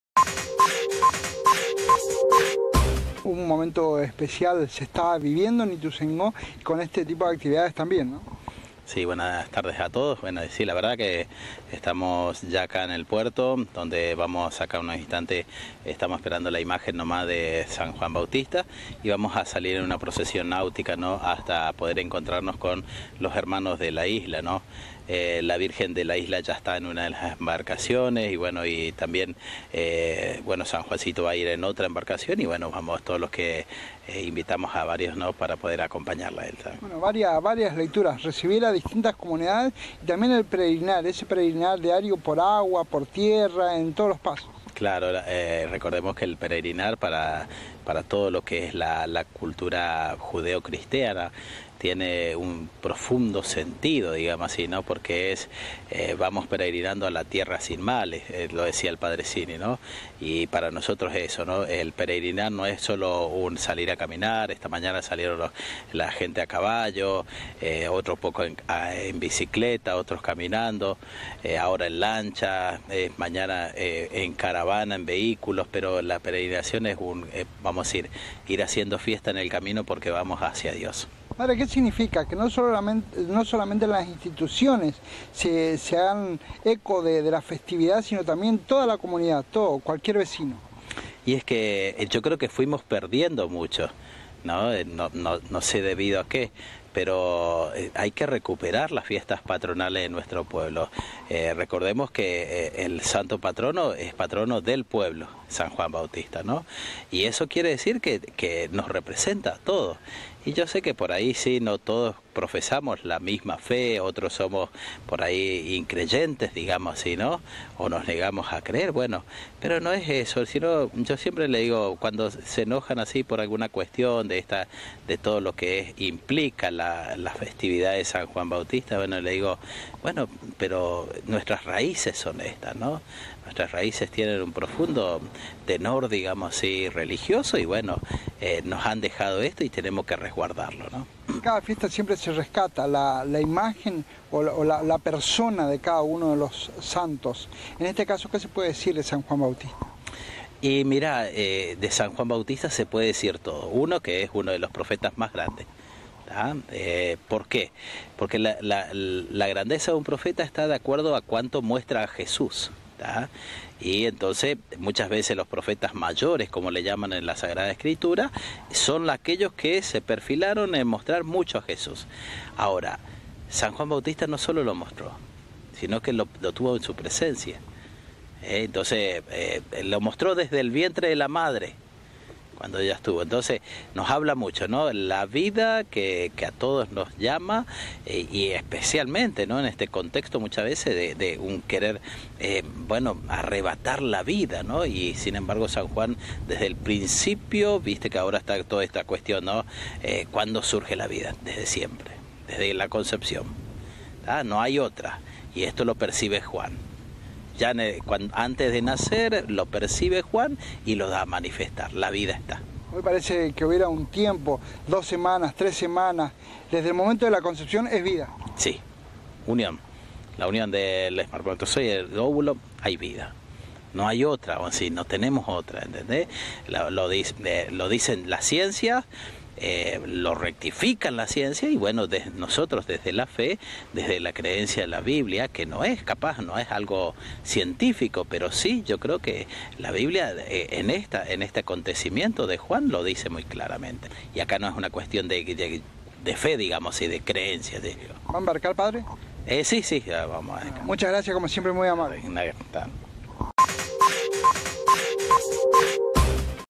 24 de Junio - Procesión Náutica en la novena y fiesta patronal de San Juan Bautista
Padre Carlos López - Cura Parroquia San Juan Bautista